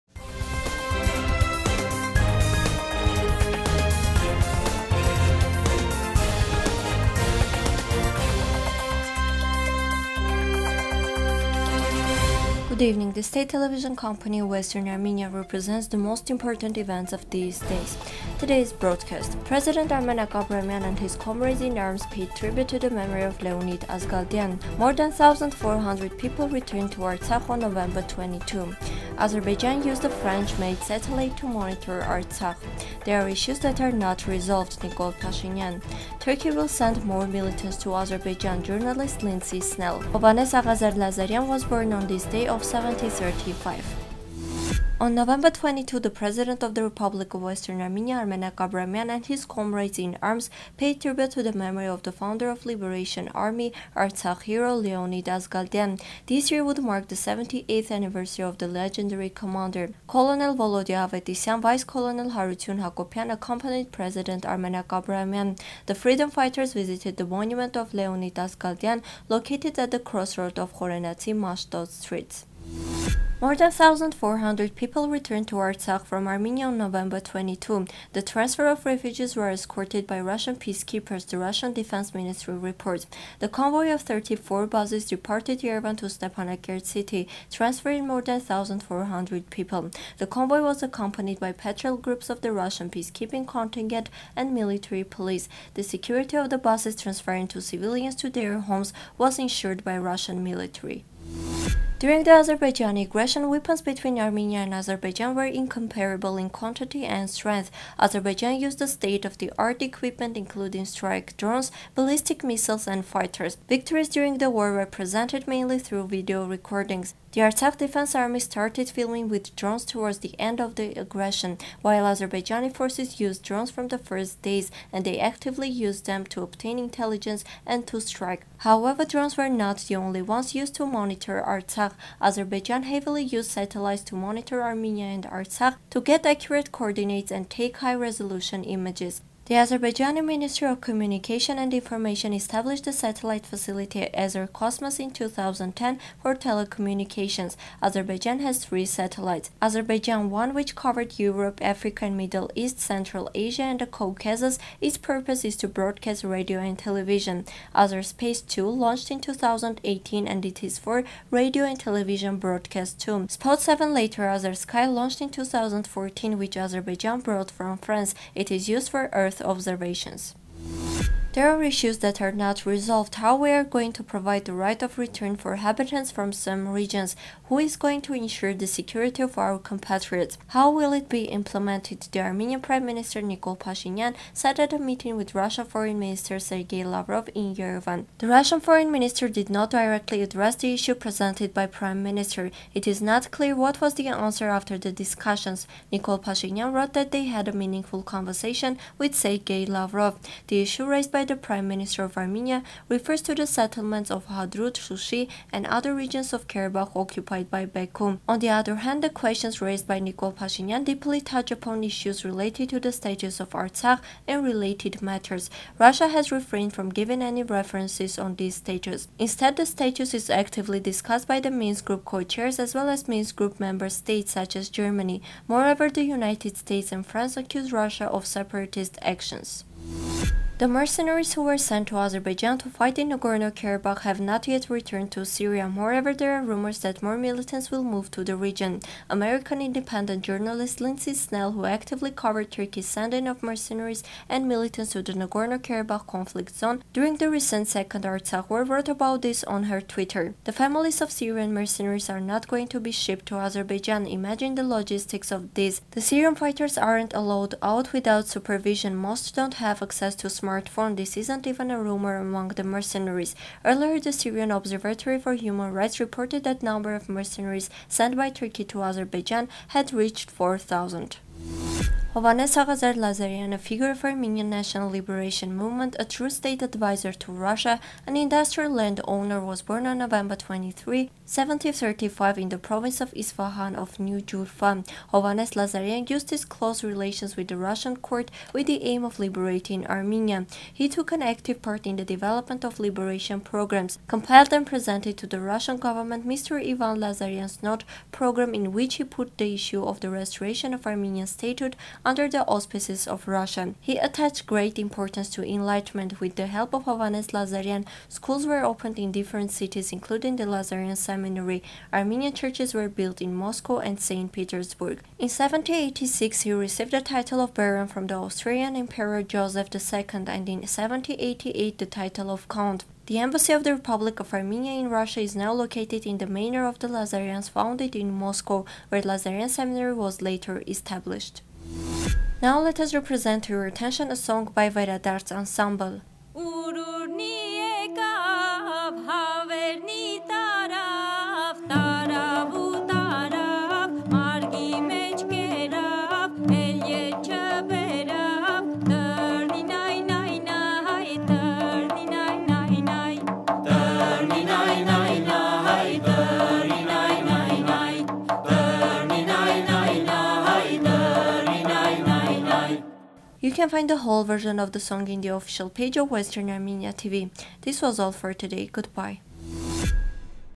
Turkey will send more militants to Azerbaijan։23.11.2020
1․President Armenak Abrahamyan and his comrades-in-arms paid tribute to the memory of Leonid Azgaldyan
2.More than 1400 people returned to Artsakh on November 22
3.Azerbaijan used a French-made satellite to monitor Artsakh
4․“There are issues that are not resolved”, Nikol Pashinyan
5.Turkey will send more militants to Azerbaijan։ Journalist Lindsey Snell
6. Hovhannes Aghazar Lazaryan was born on this day of 1735